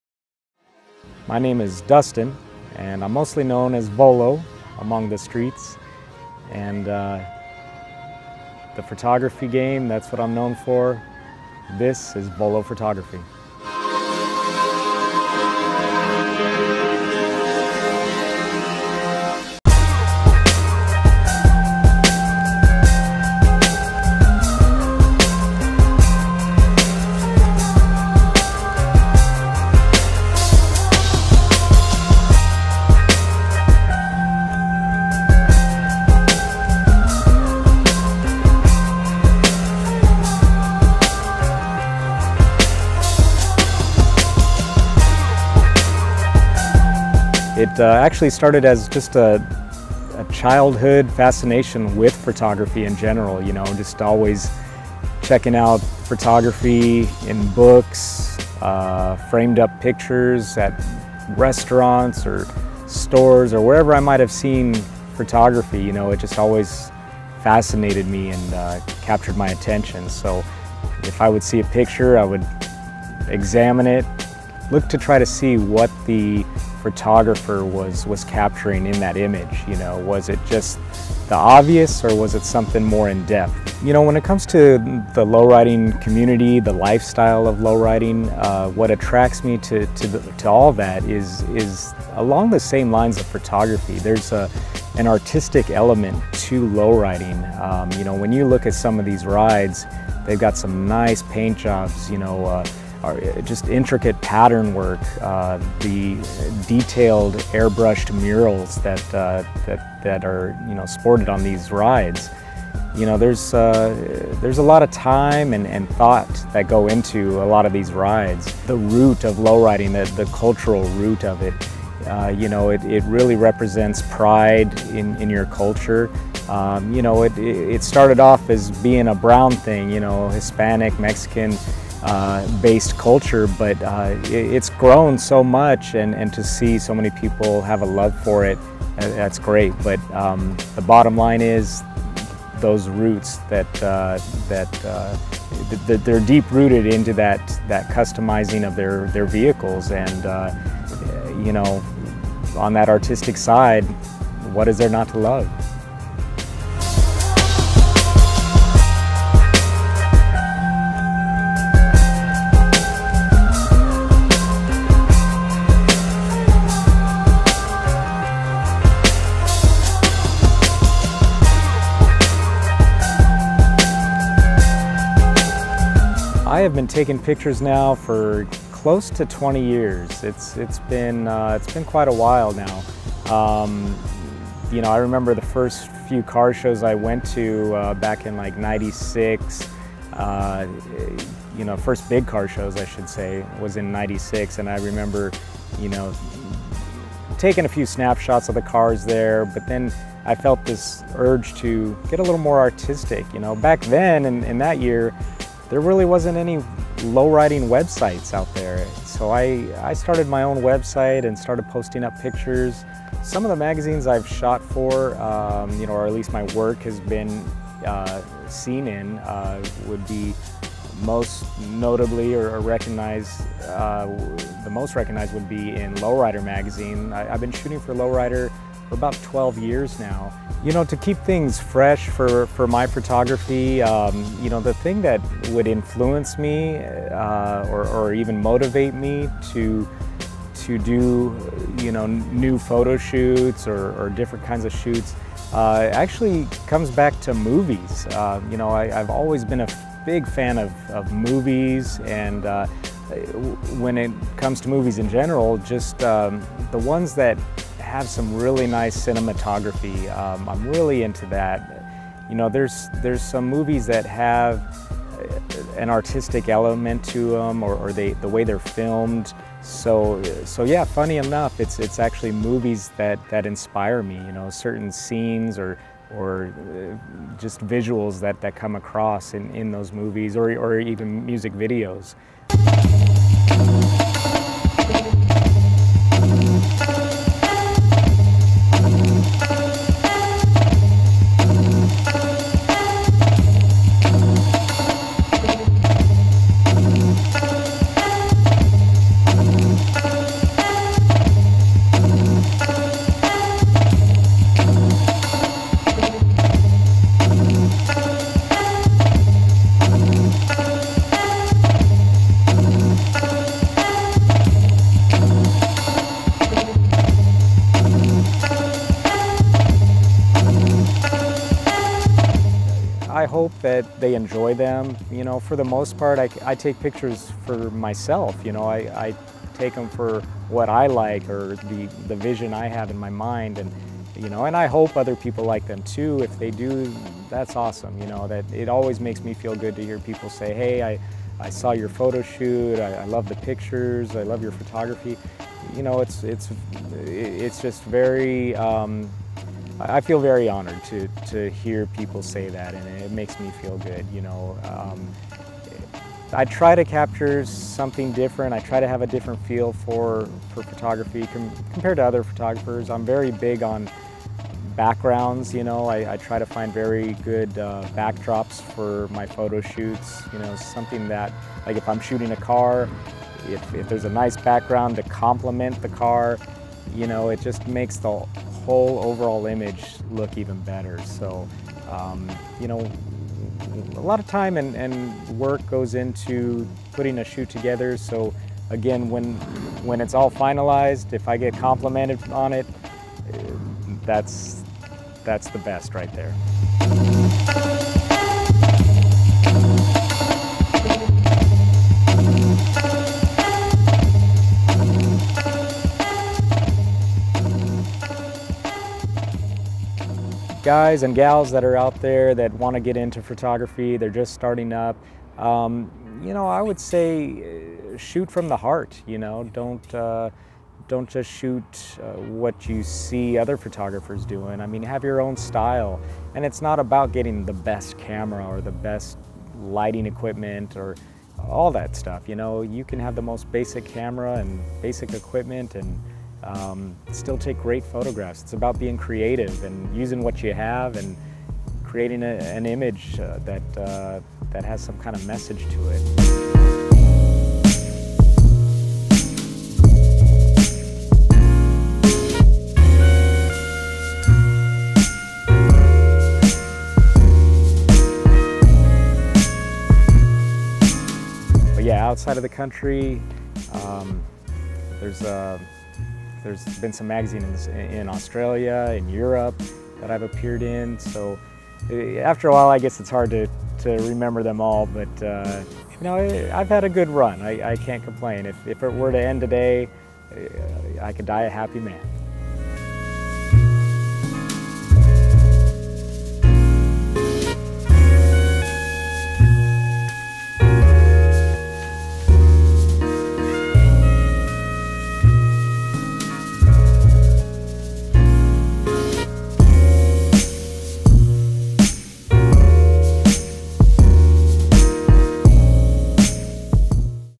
Beyond the Arts Presents: Volo Urban Photography
Beyond the Arts highlights some of our favorite artists. On this episode we get to talk to Volo, one of the top Urban photographers in the game on what got him started, what motivates him, and much more.